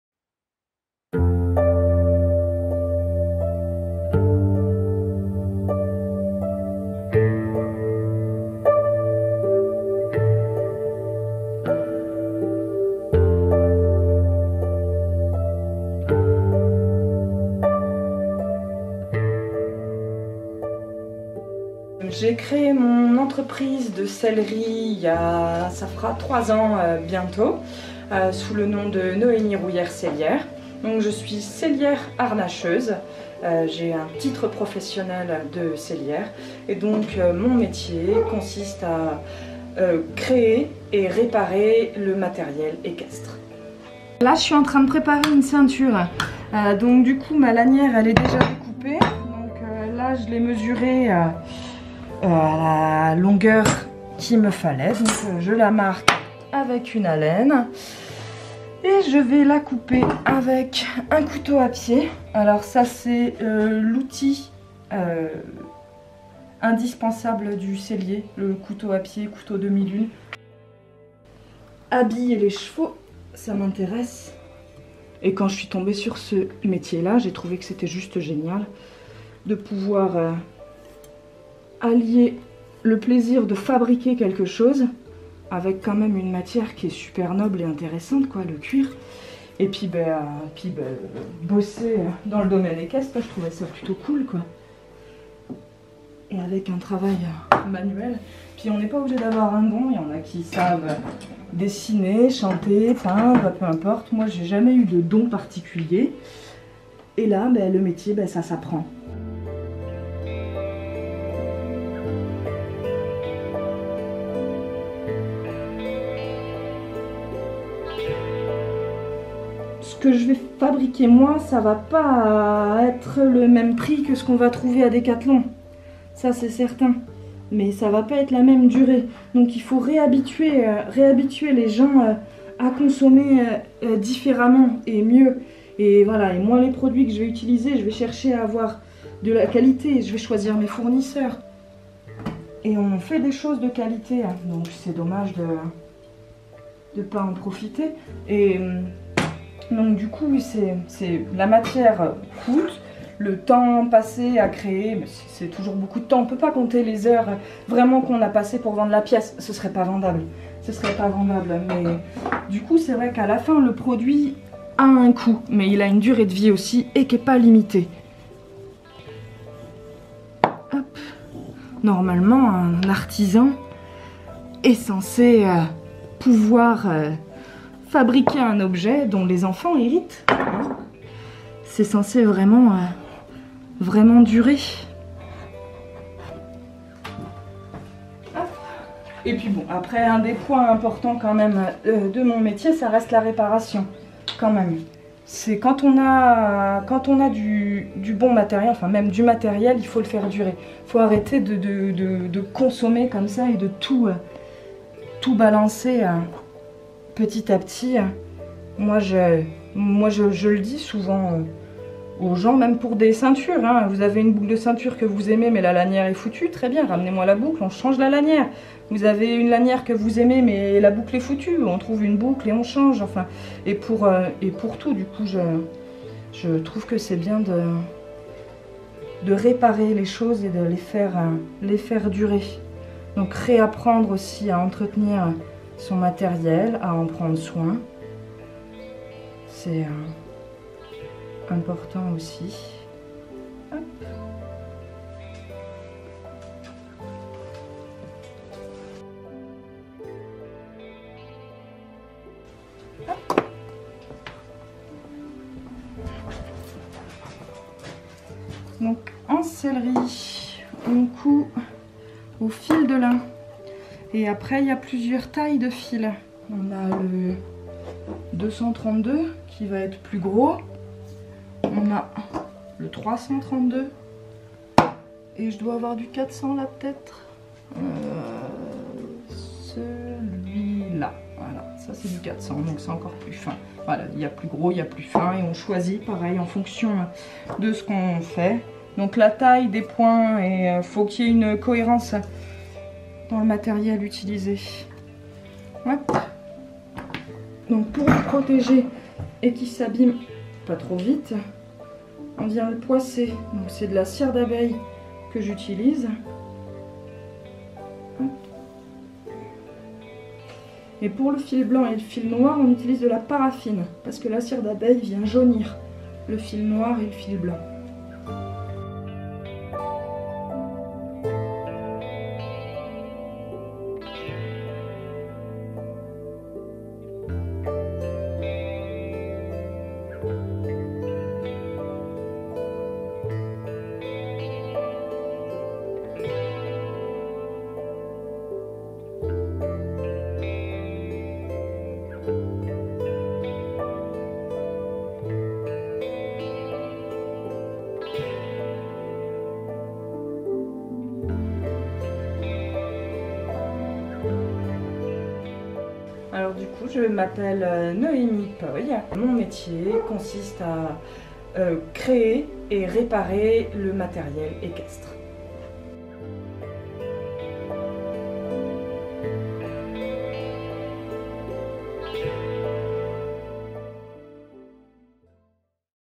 Le métier de Sellier - Harnacheur avec Noémie Rouyère Sellière
Noémie Rouyère est sellière-harnacheuse dans le Lot, sur la commune de Livernon. Sa passion : habiller les chevaux de ses belles créations en cuir : licols, filets, bridons, sacoches, selles...

Elle revient avec nous sur son parcours, le travail du cuir, sa vision de l'artisanat aujourd'hui et son rôle important dans un monde où l'on a trop tendance à jeter plutôt qu'à réparer ce qui pourrait l'être.